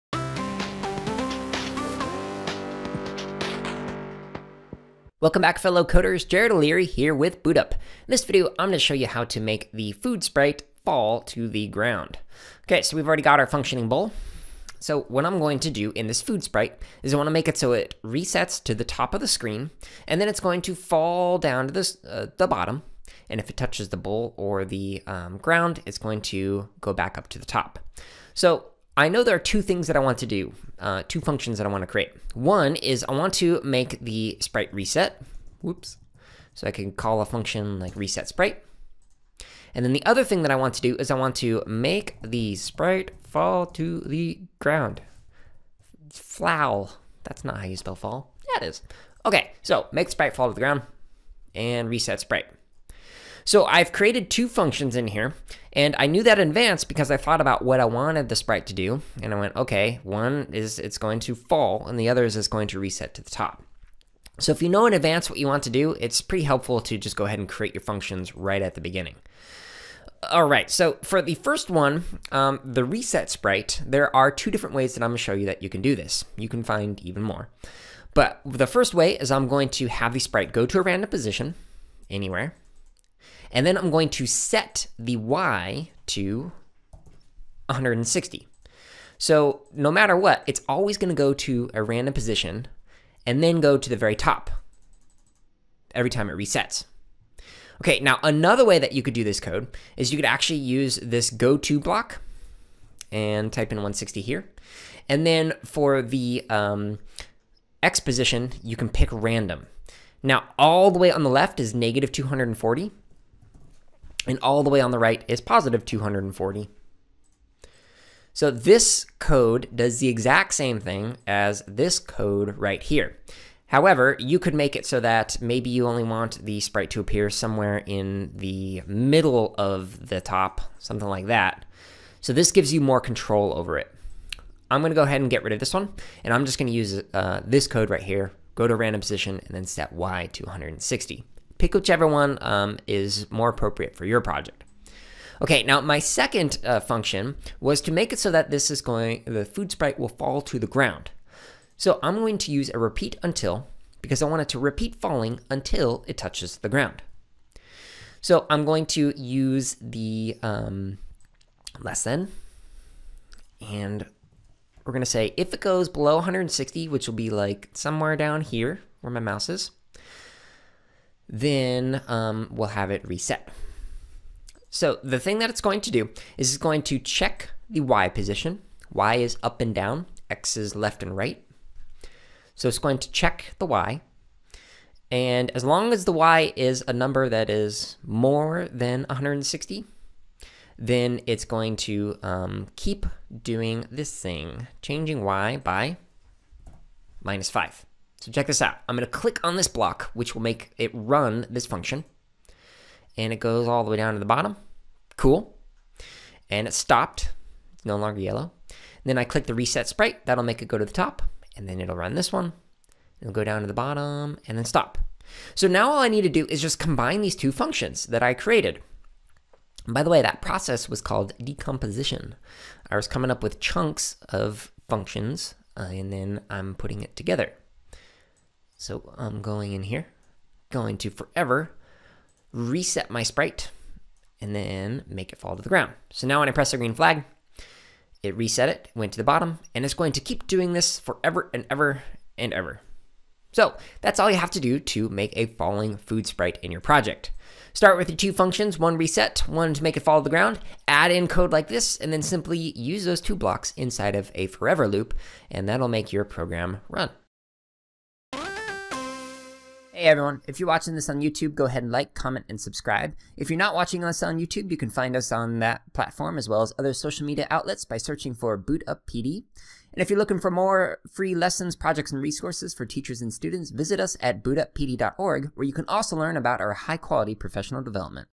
Food Catcher - Make the Food Sprite Fall
Coders create a food catcher game that keeps track of the player’s score. The purpose of this project is to introduce variables to keep track of a player’s score.